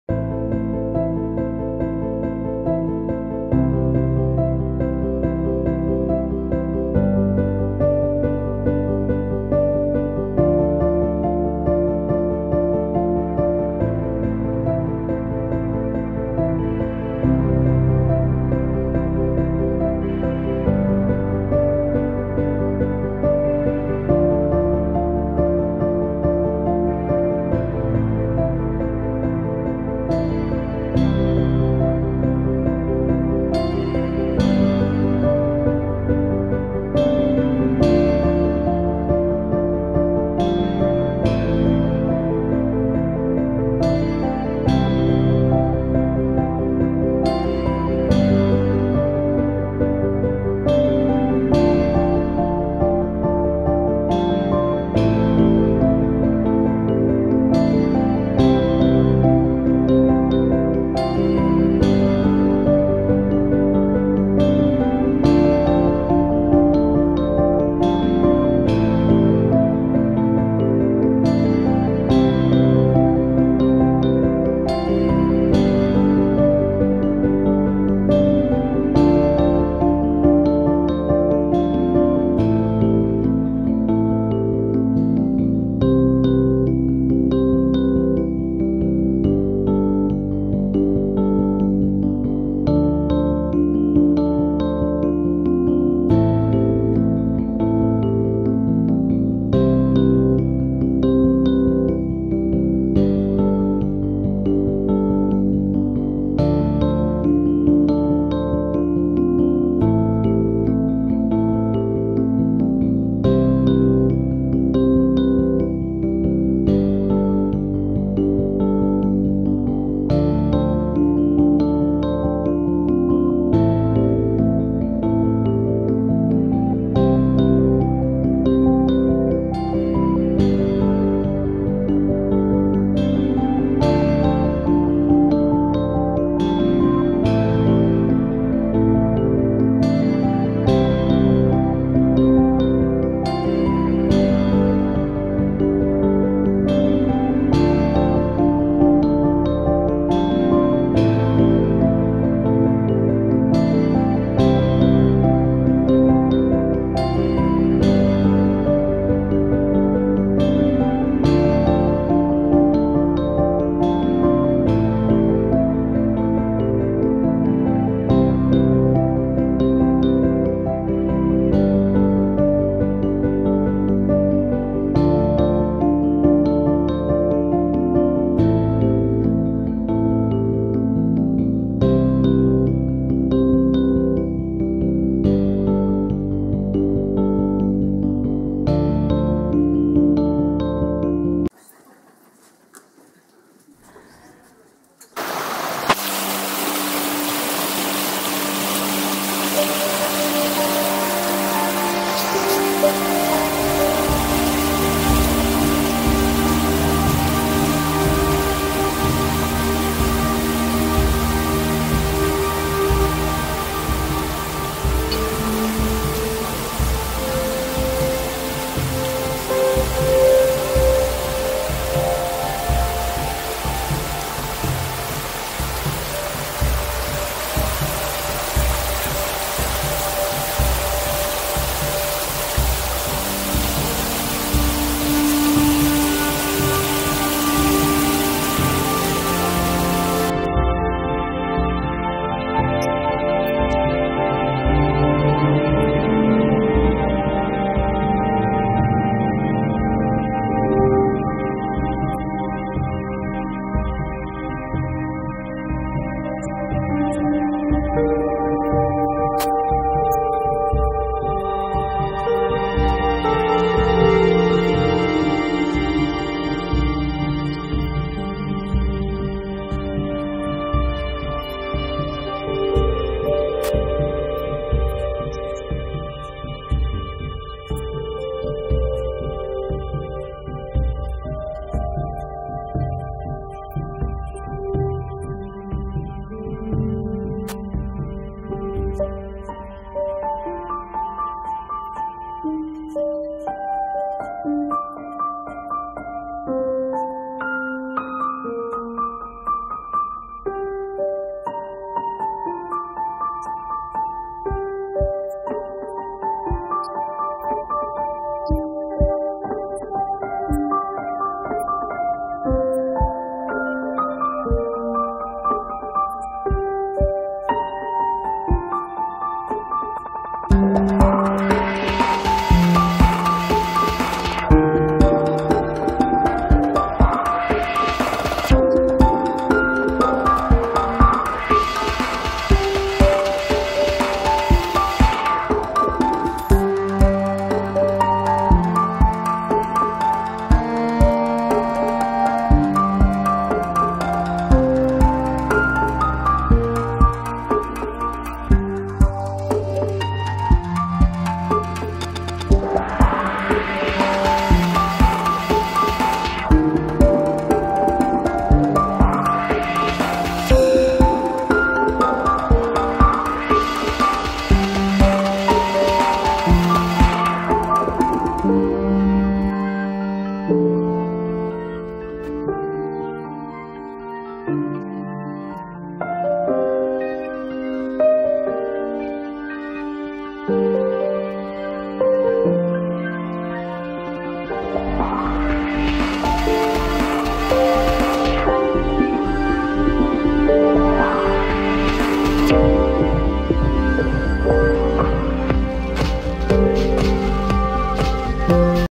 El gorgo de la escalera Anna Valencia VR 360
Anna, un pueblo a 68Km de Valencia con varios parajes naturales dignos de visitar.
Sumérgete en la realidad virtual con gafas VR o moviendo tu móvil, contempla el Gorgo de la Escalera.